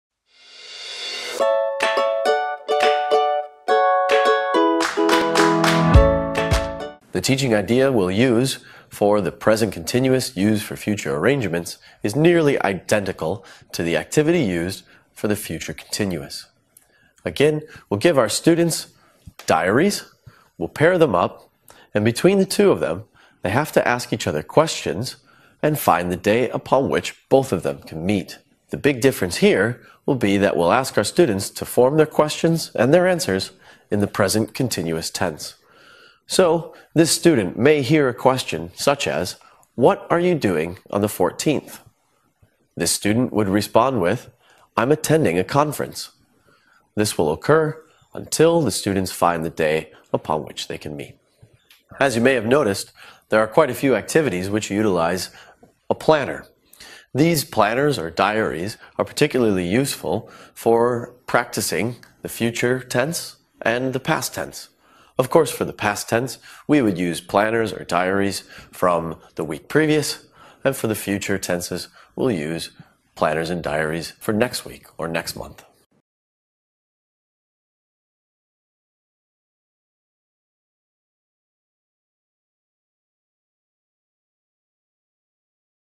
The Future Tenses - Other Future Forms - Present Continuous - Teaching Idea
This teaching idea is very similar to the activity we can use for the future continuous. We pair up our students, give them diaries and have them find the day on which both of them are free to meet. This video shows you exactly how to execute this activity in the classroom.